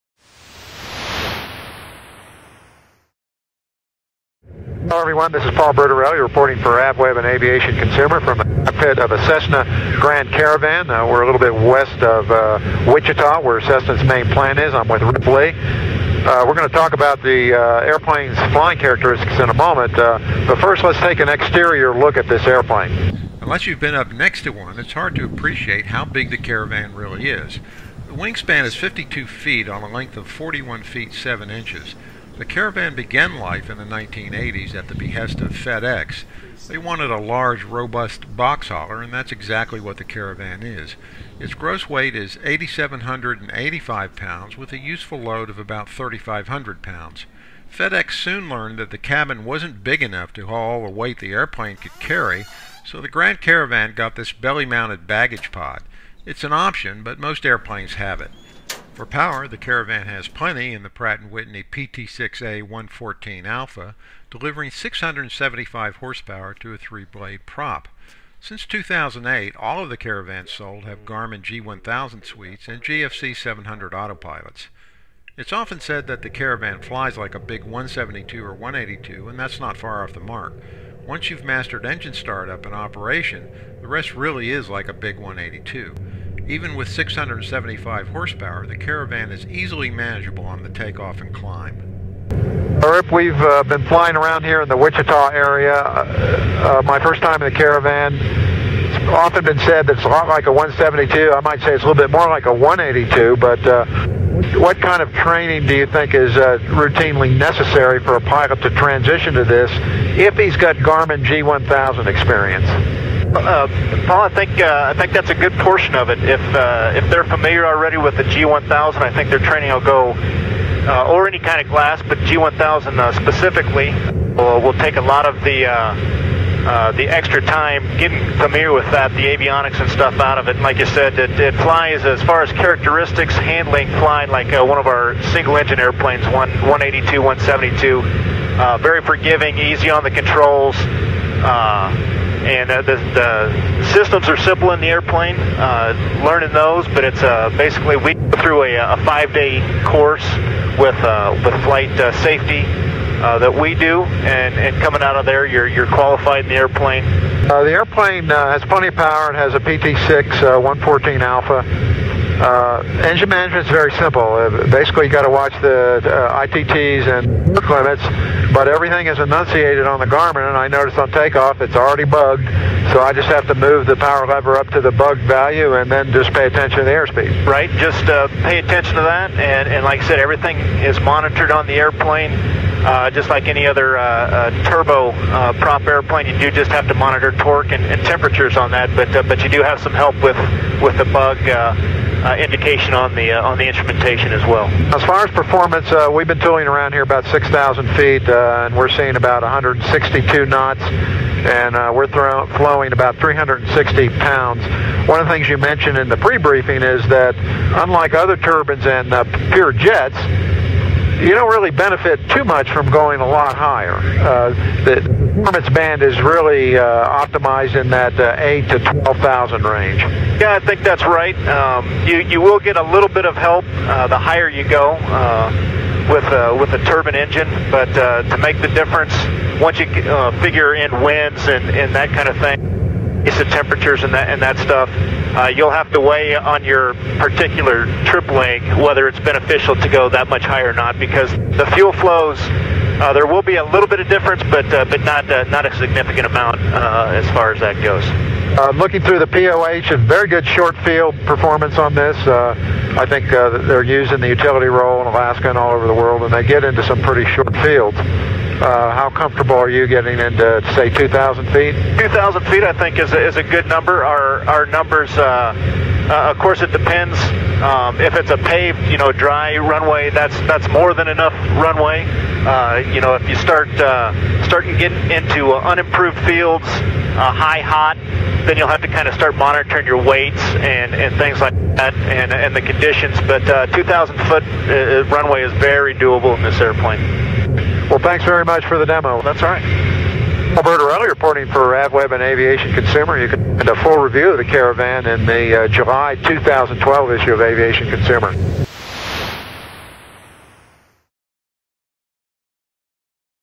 Cessna Caravan Flight Demo
Since its launch in 1985, the Cessna Caravan and later the Grand Caravan have been strong sellers for Cessna.  In this video, AVweb takes a flight demo in the latest model, complete with G1000 and GFC700 autopilot.